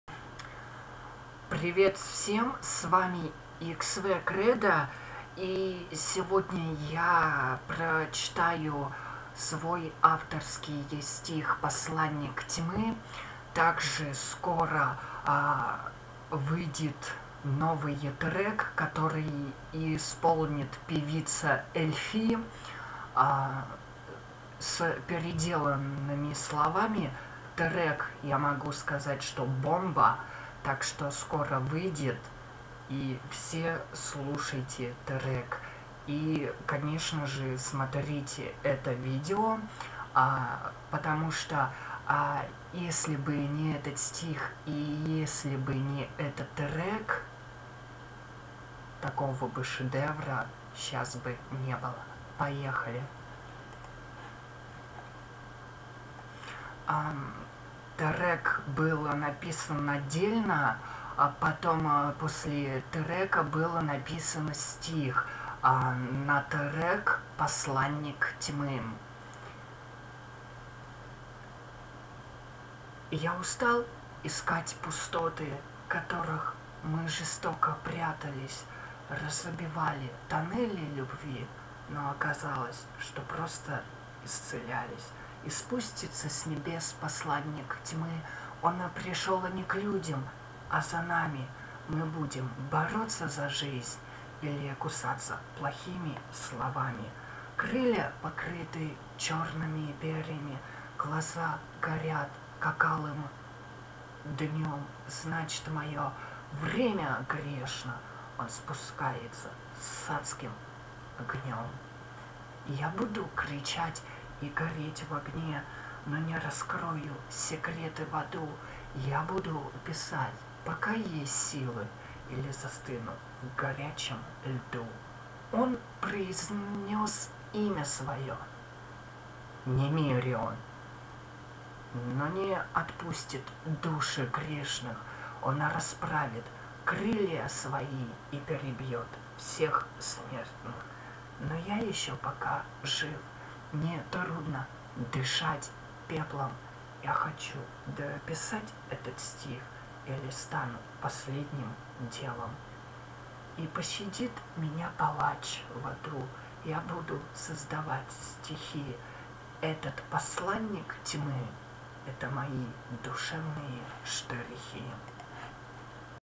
Посланник тьмы.
Я читаю свой стих: "Посланник тьмы".